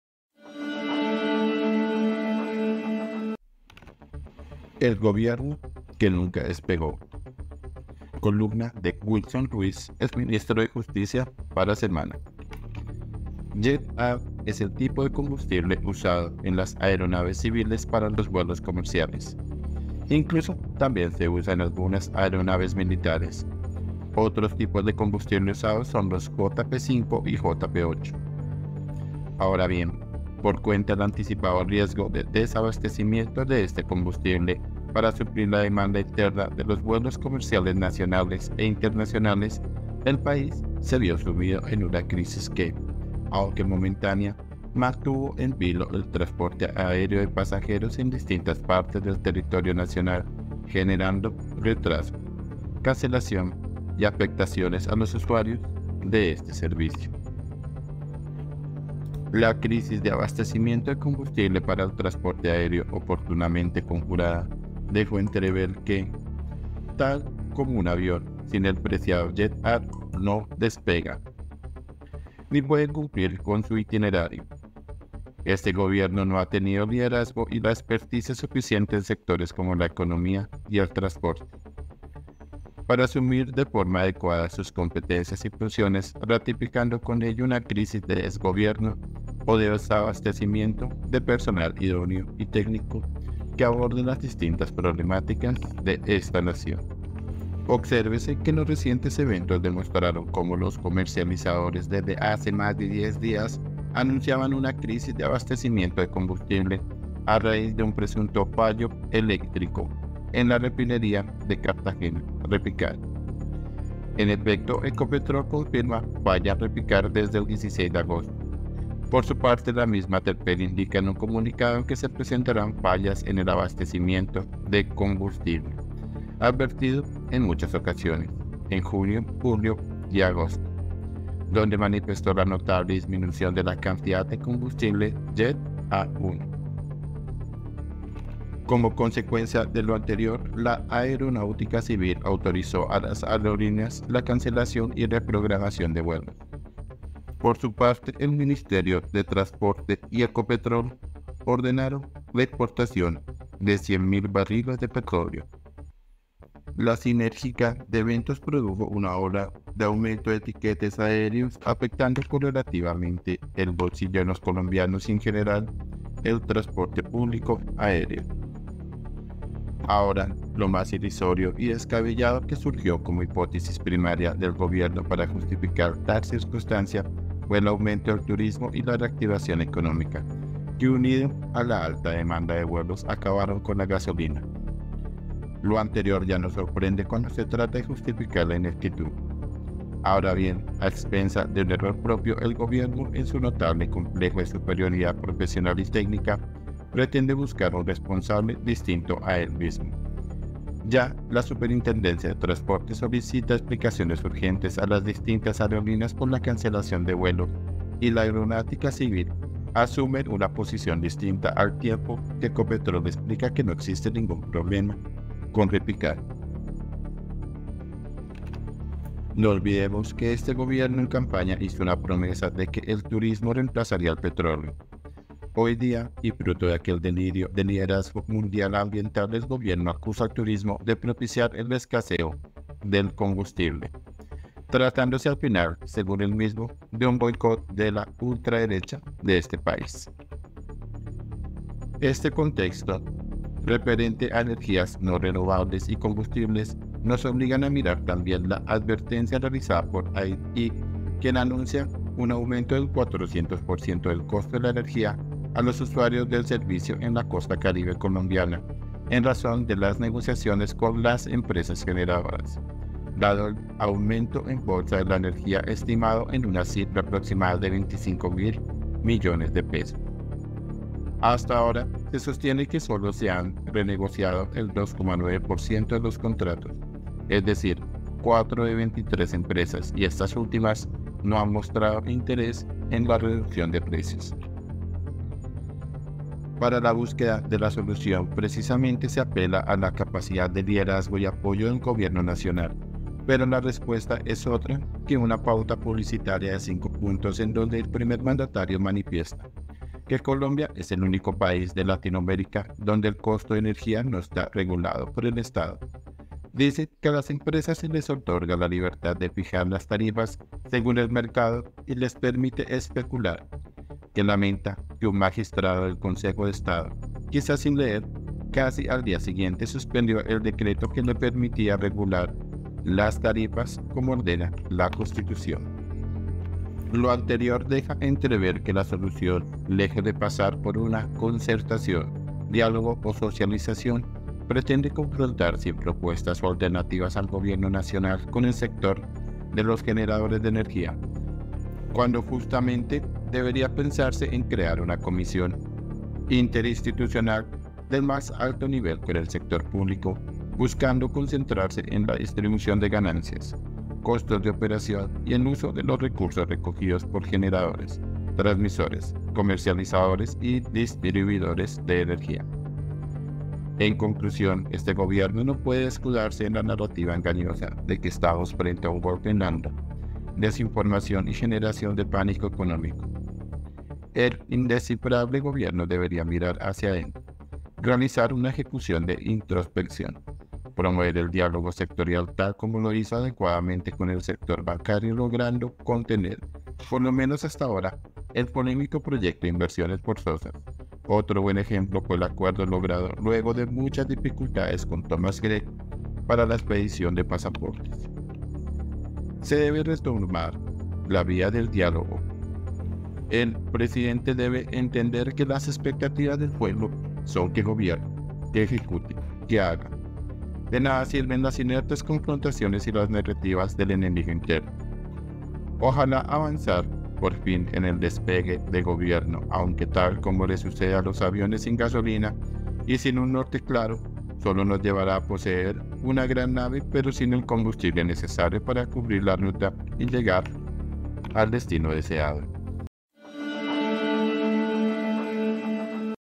EL GOBIERNO QUE NUNCA DESPEGÓ 🔴 Columna Wilson Ruiz
Jet A es el tipo de combustible usado en las aeronaves civiles para los vuelos comerciales, incluso también se usa en algunas aeronaves militares, otros tipos de combustible usados son los JP-5 y JP-8. Ahora bien, por cuenta del anticipado riesgo de desabastecimiento de este combustible para suplir la demanda interna de los vuelos comerciales nacionales e internacionales, el país se vio sumido en una crisis que, aunque momentánea, mantuvo en vilo el transporte aéreo de pasajeros en distintas partes del territorio nacional generando retrasos, cancelación y afectaciones a los usuarios de este servicio.
columna WILSON RUIZ para SEMANA